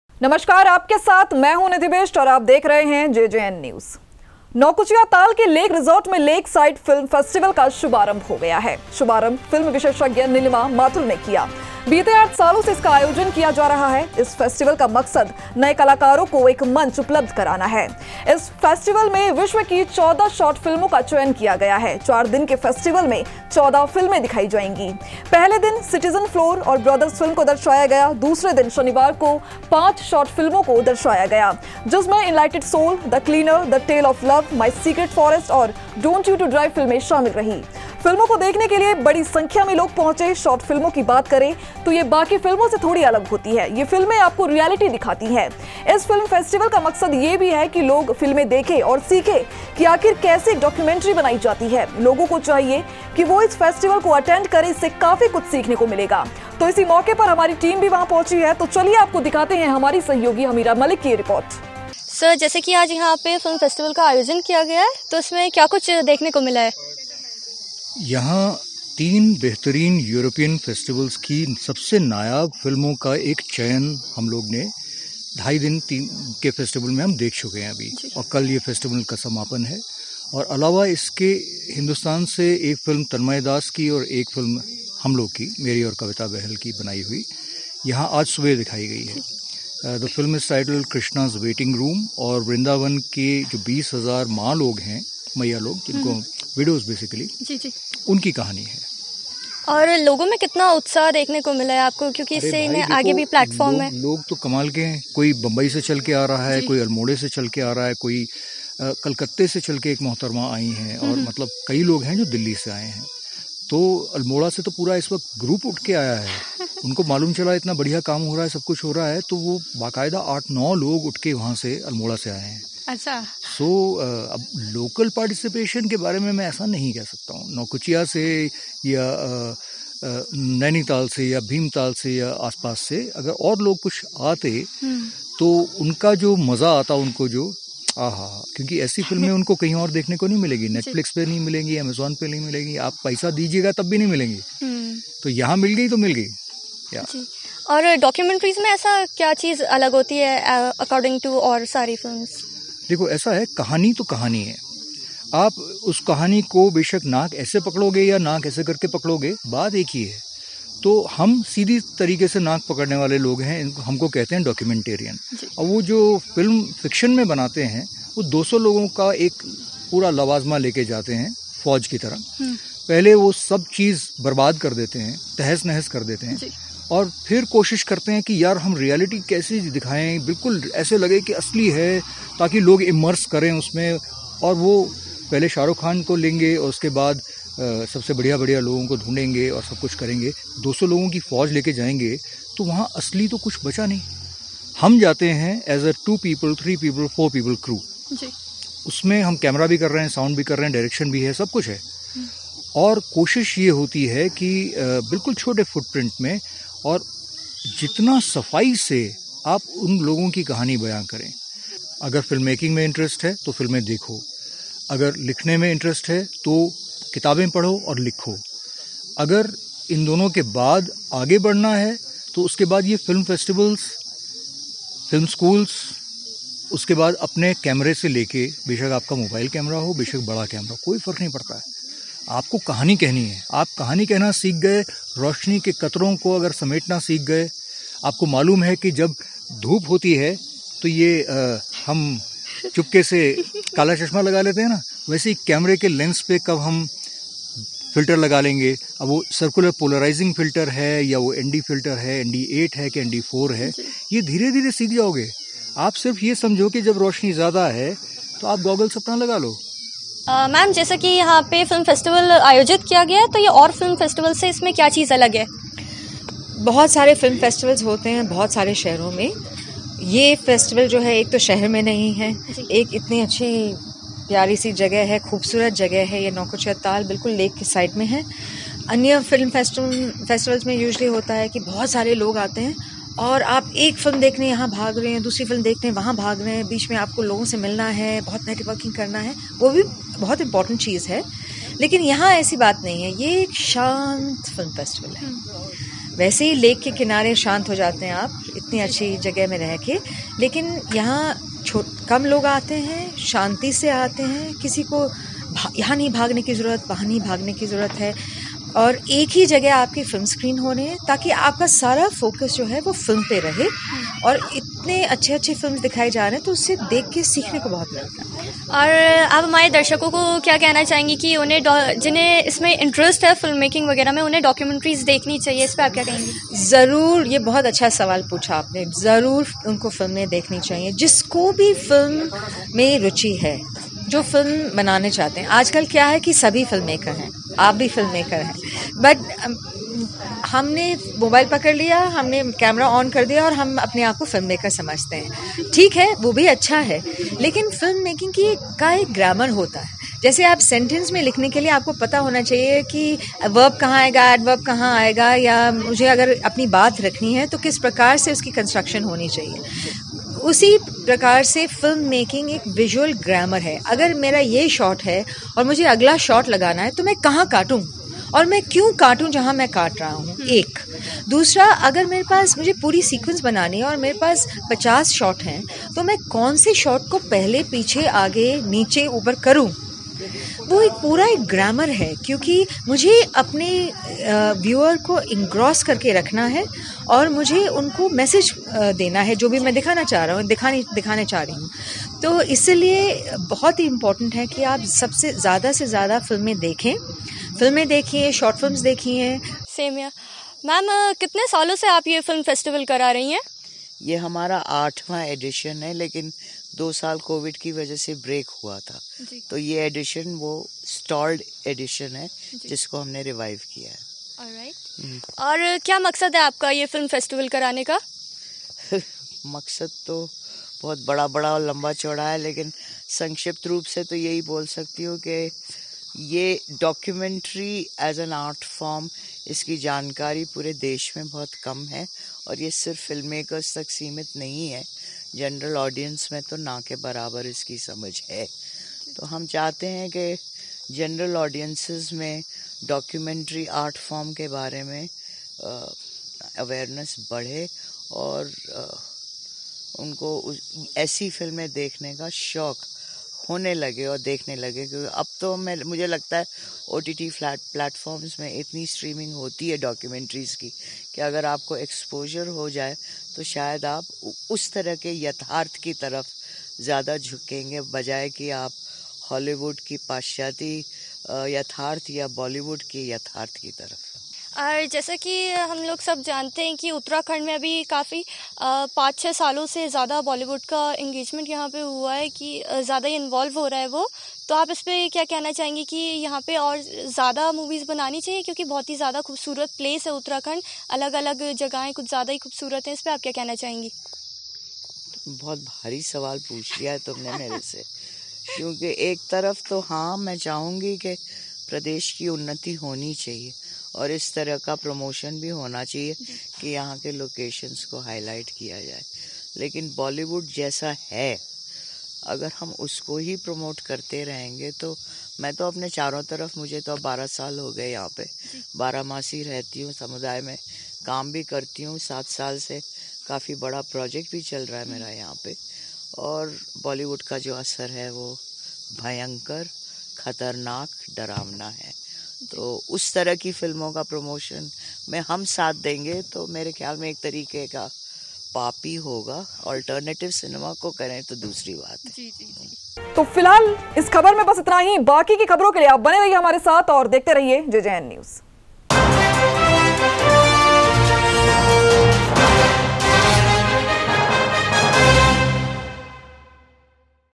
नौकुचियाताल में 4 दिवसीय फिल्म फेस्टिवल का लुफ़त उठाने पहुंची जनता || JJN News
JJN News Nainital,
JJN News,